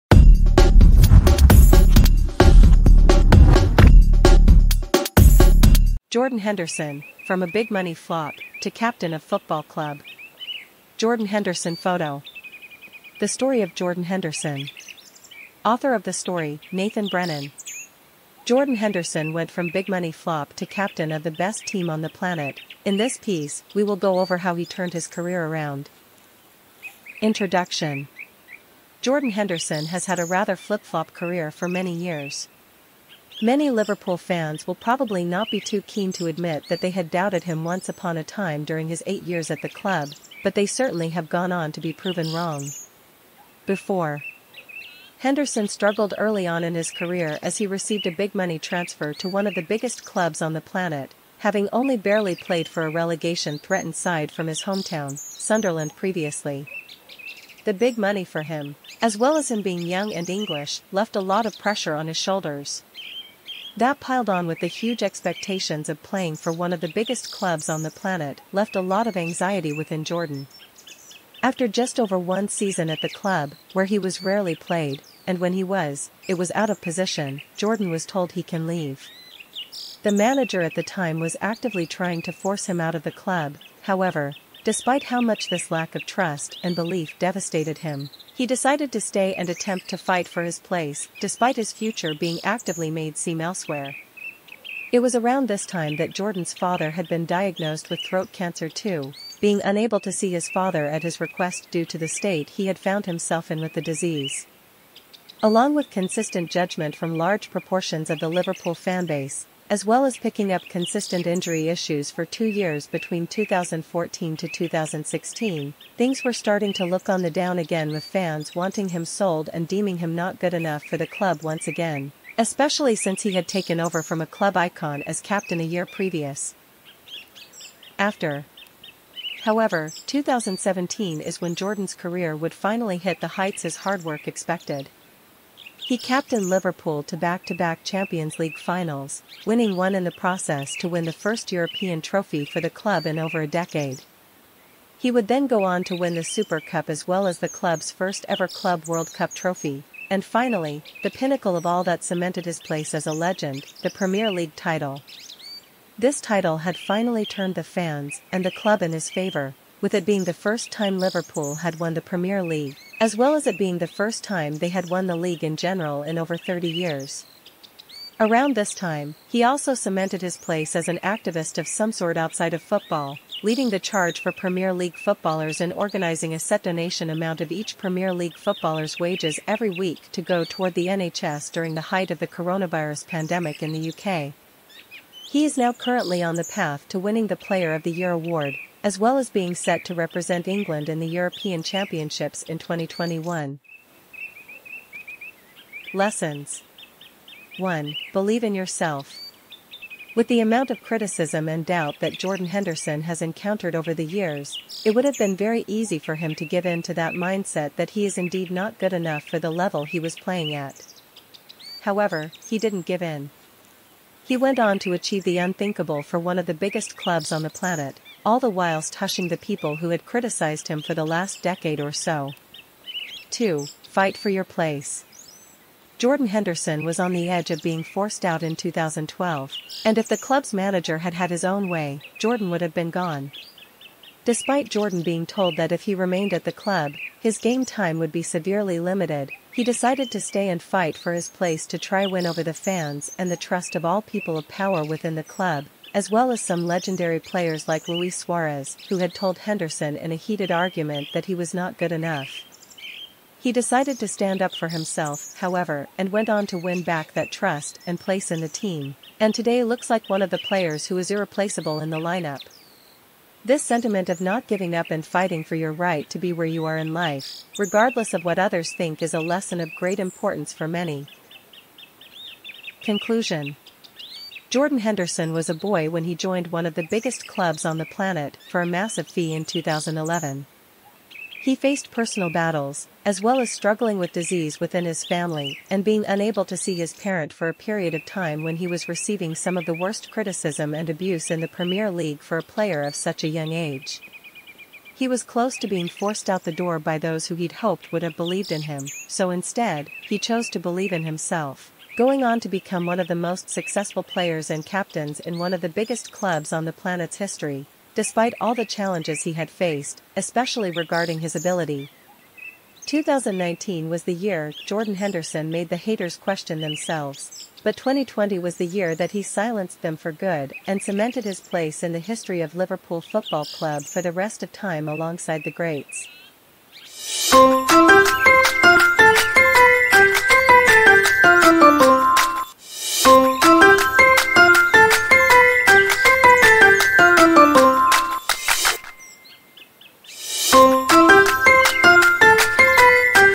Jordan Henderson: Triumph of a Captain
Witness the remarkable journey of Jordan Henderson, a story of resilience, determination, and ultimate triumph. Once labeled a "big-money flop," Henderson defied the odds to emerge as the formidable captain of his football club. Through unwavering dedication and sheer grit, he transformed setbacks into stepping stones, proving doubters wrong with every stride on the pitch. Experience the evolution of a true leader, whose journey inspires and captivates, reminding us that success is not just about talent, but about the relentless pursuit of excellence. Join the legacy of Jordan Henderson, where failure is merely a pit stop on the road to greatness.